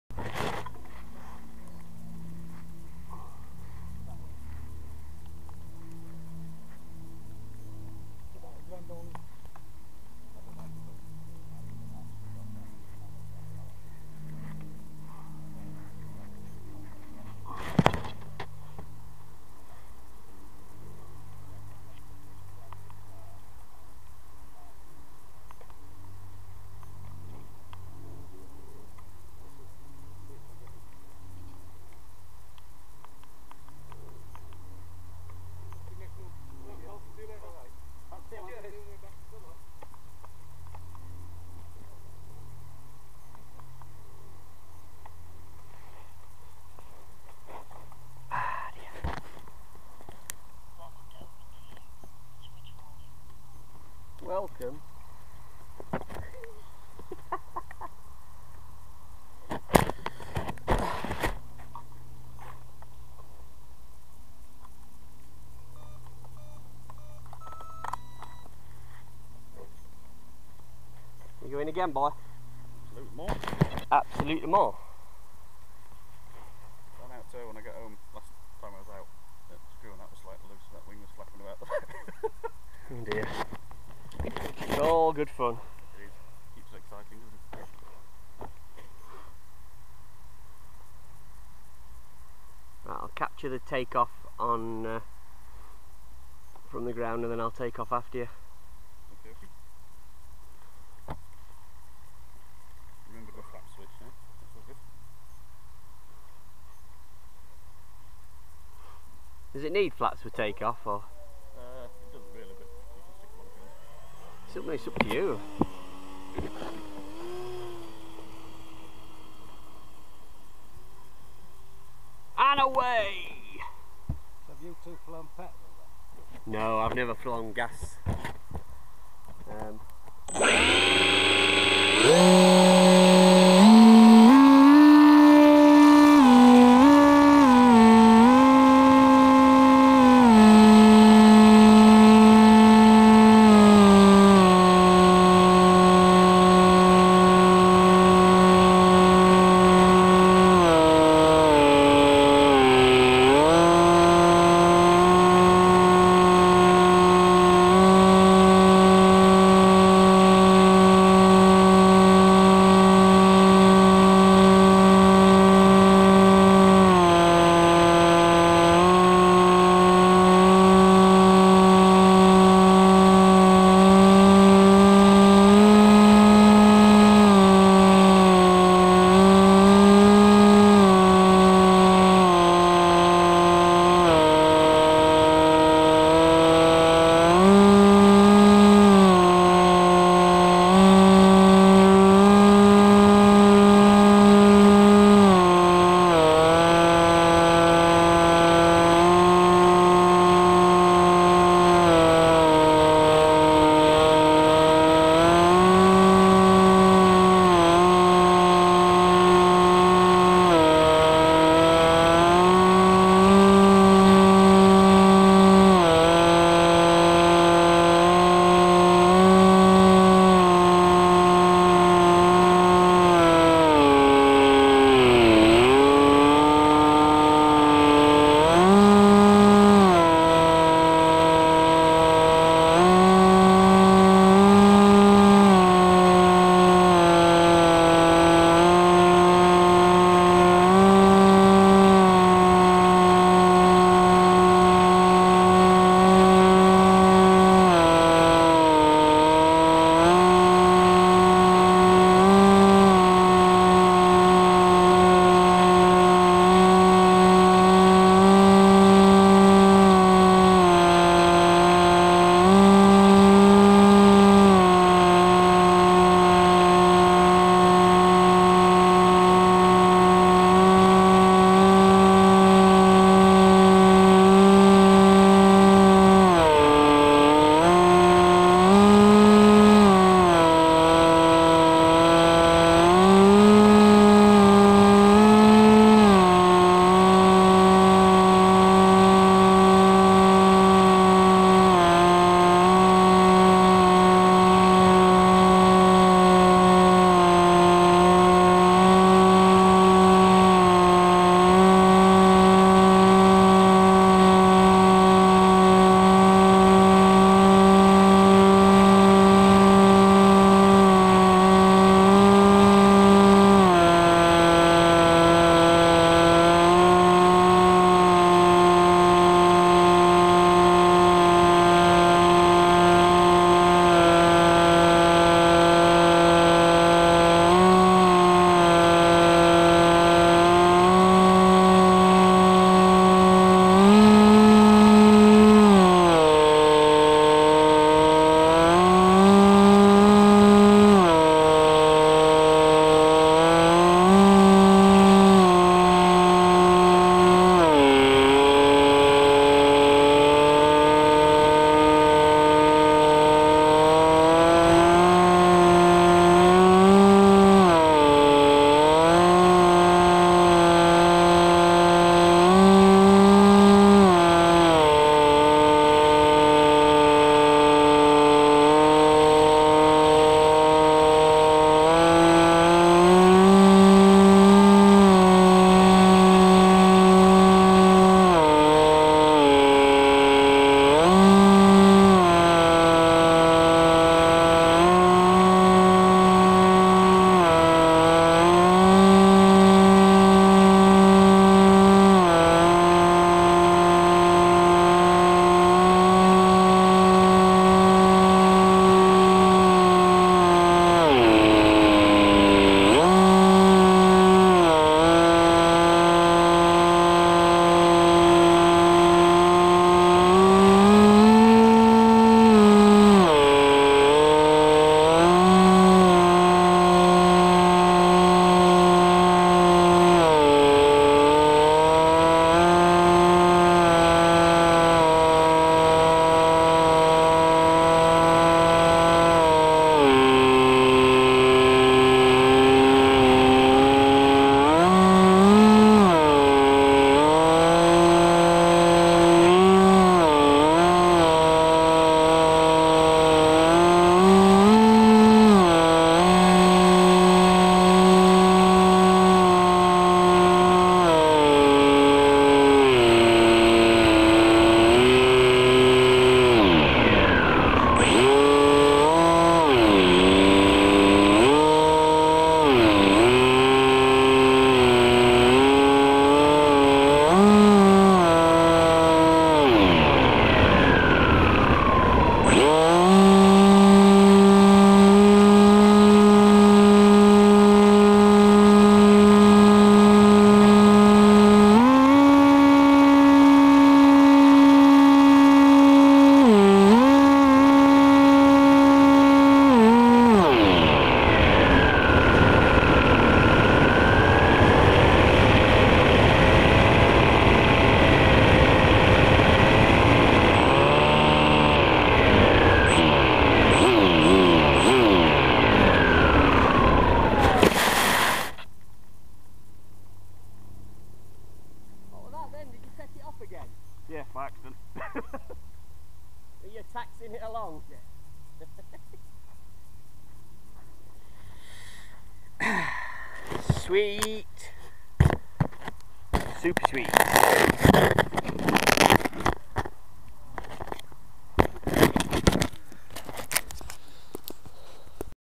TekSumo FPV Following Adrian's Cessna
Perfect weather yesterday, so ideal conditions for the TekSumo to get some good footage following a few of Adrian's Planes.
This time, his Cessna. Not sure of the Brand, but he's had it a while & in the calm conditions it flew great and Adrian even managed a couple of perfect landings. Very hard on soft ground with long grass & the Tri-Landing gear!!!